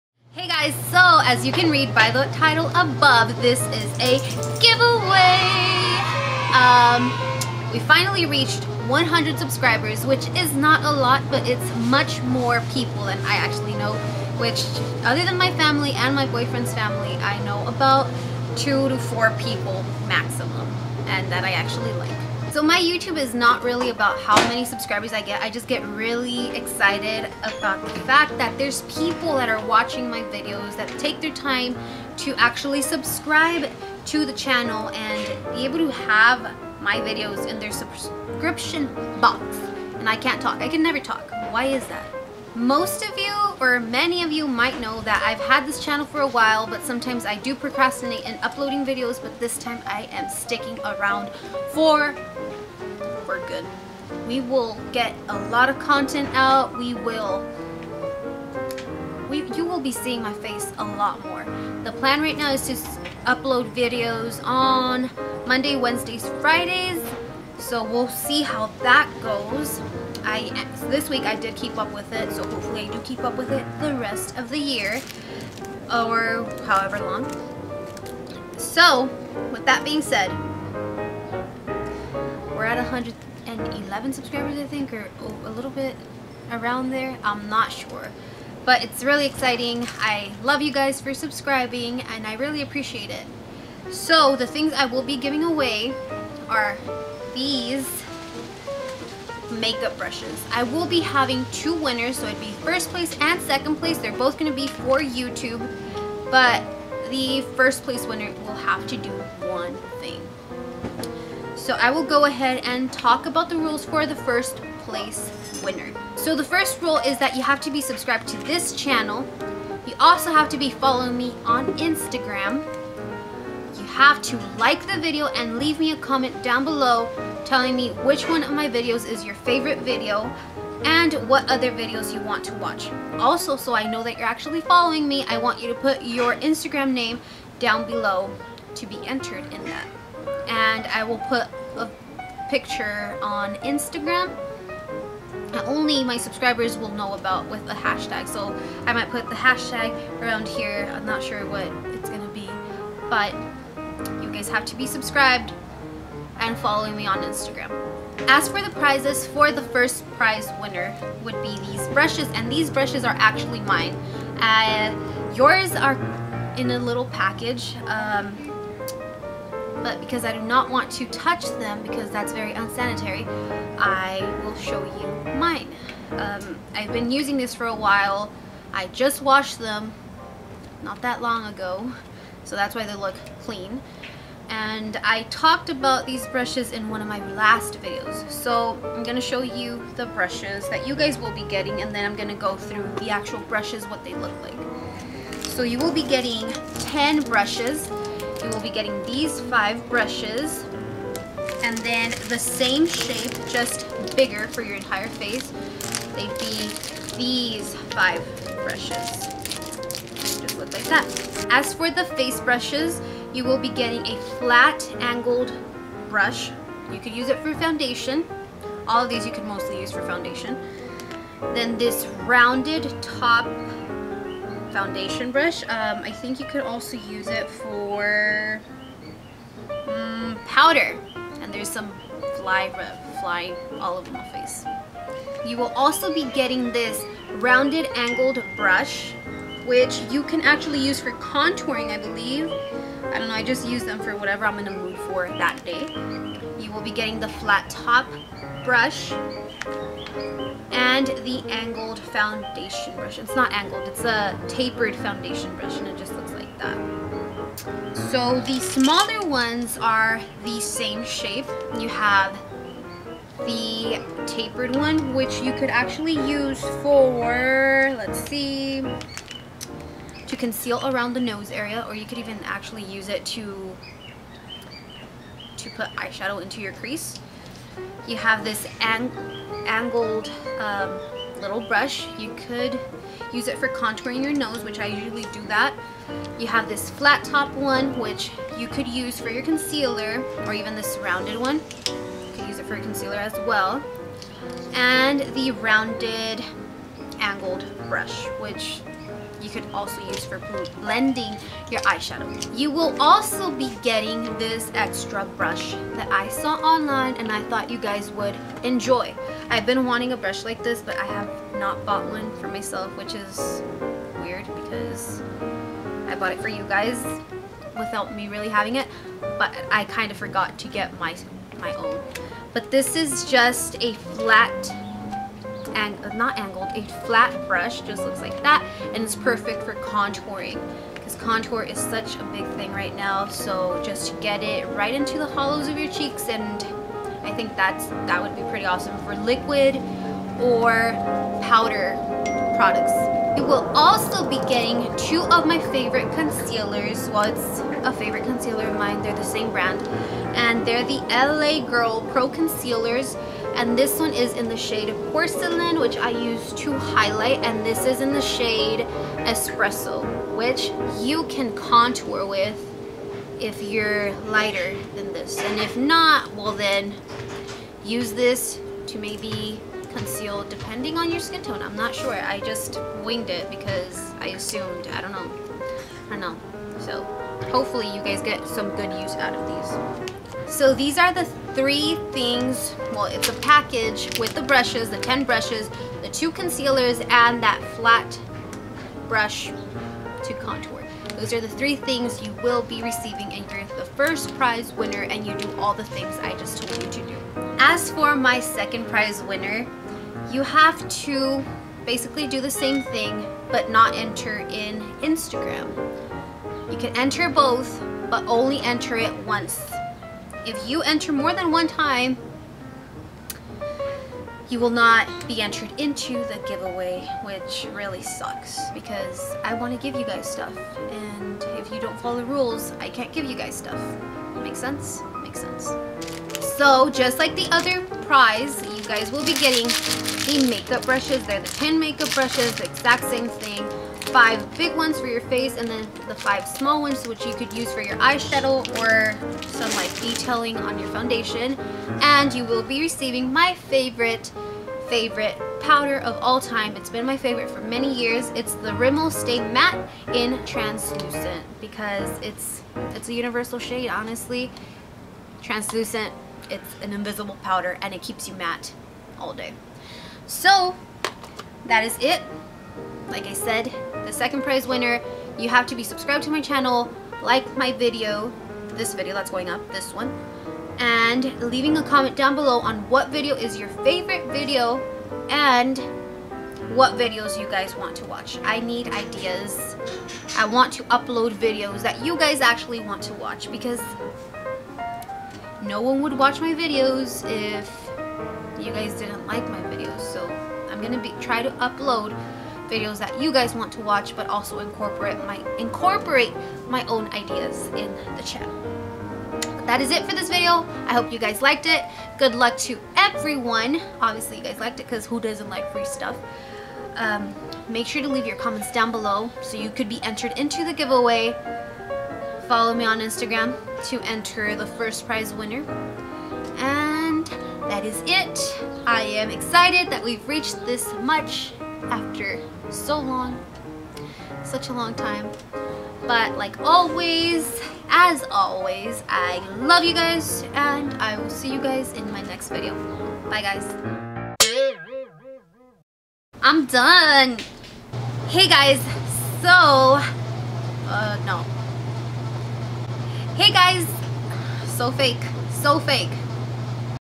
Mini Giveaway 2016!!! 100 Subscriber Giveaway!!!(closed)
Expand the box for more info...

Not International... Sorry..

1st Prize Winner Rules:
3. Like this video and Comment telling me which video is your favorite and what videos you would want to see.
4. 100YTSubbies on Instagram with a picture of their favorite makeup product.

2nd Prize Winner:
3. Like this video.